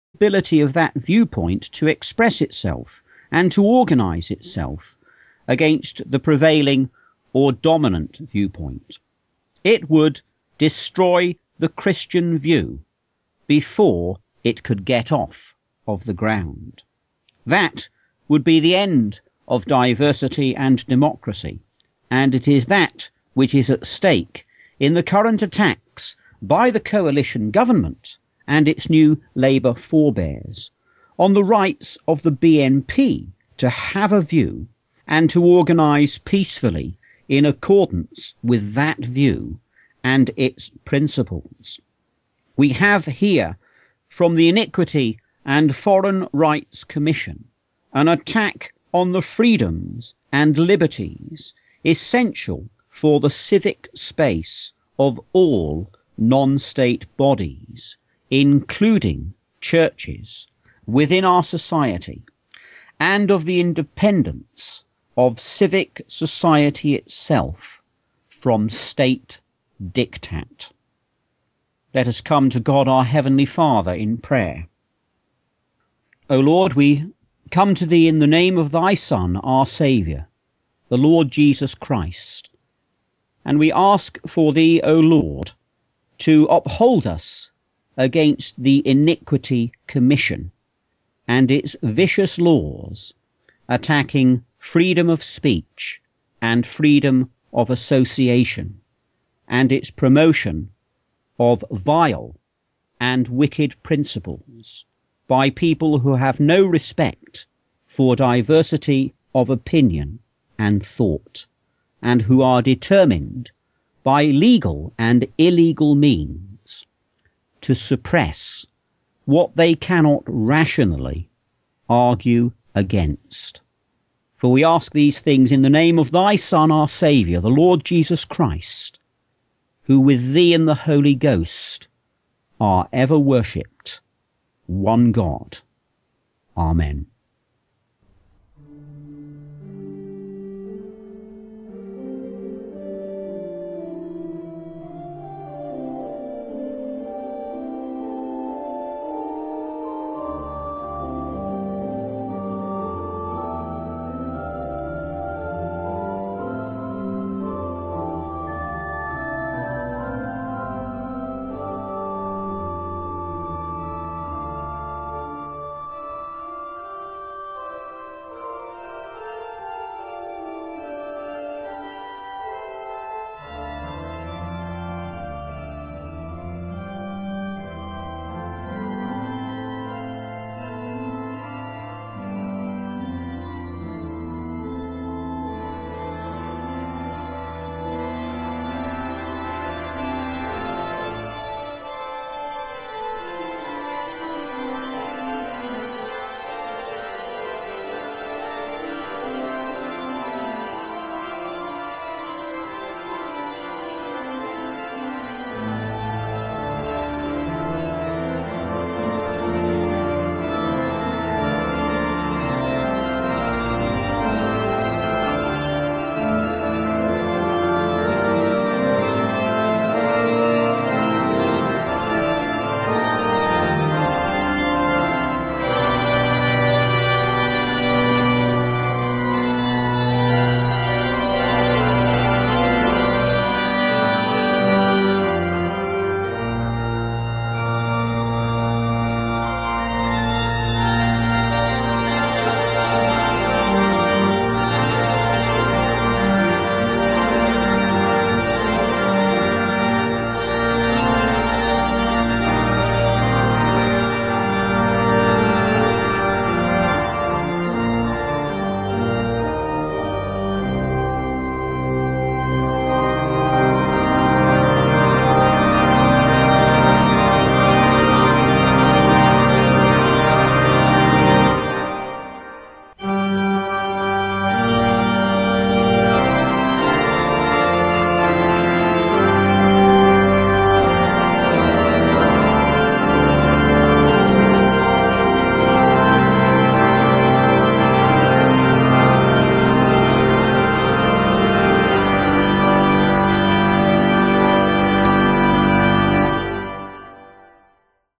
BNP's Reverend West on Radio RWB: "Persecution Sunday" - 3/3
The attempt by the Equalities and Human Rights Commission to crush the British National Party is an assault on freedom of association which will negatively impact on all Christians, regardless of their political beliefs, the Reverend Robert West has said.

Speaking in his regular Sunday sermon broadcast on Radio Red, White and Blue, Reverend West said the real objective of the EHRC's attack on the BNP was "a political one of oppression of a political party and the dispossession of a people.

"What is crucial for Christians to understand is that this is also done in the context of a media, legal and government campaign against Christians and their views and values," he said.

"There is a unifying thread in all this: the moral law which is under attack by equality/iniquity legislation, in the case of Christians and homosexuality, is also to be the victim of state bodies which now seek to promote the anti-Biblical religion of Islam and to attack the Biblical and Judeao-Christian teaching of nations and nationalism; the preparation for this being the equating, with little justification, of nationalism — especially British nationalism — with racism.

"This word, racism, is being used as a bullying and brainwashing term to stop British people defending their right to a national home in their own country in the face of what is an ongoing scandal: the invasion and colonisation of their country by mass and unwanted levels of foreign immigration," Revd West continued.

"In like manner the word, Islamohphobia, has been invented to beat down rational thought and cogent analysis of the violent and anti-democratic nature of core Islamic texts and their mainstream sharia.

"It is in trying to becloud the issues which are at stake that the Iniquity and Inhuman Rights Commission is seeking to do its evil work on Britain and our British democracy and way of life. We all stand to lose if it gains its wicked way."

He went on to point out that the EHRC's attempt to prescribe to the BNP who its members could be and what it believes in, "would mean the end of the ability of that viewpoint to express itself, and to organise itself, against the prevailing or dominant viewpoint."

This logic, applied to Christian churches, would "destroy the Christian view before it could get off of the ground.

"That would be the end of diversity and democracy; and it is that which is at stake in the current attacks by the coalition Government, and its New Labour forebears, on the rights of the BNP to have a view and to organise peacefully in accordance with that view.

"We have here from the Iniquity and Foreign Rights Commission an attack on the freedoms and liberties essential for the civic space of all non-state bodies, including churches, within our society, and of the independence of civic society itself from state diktat," Revd West concluded.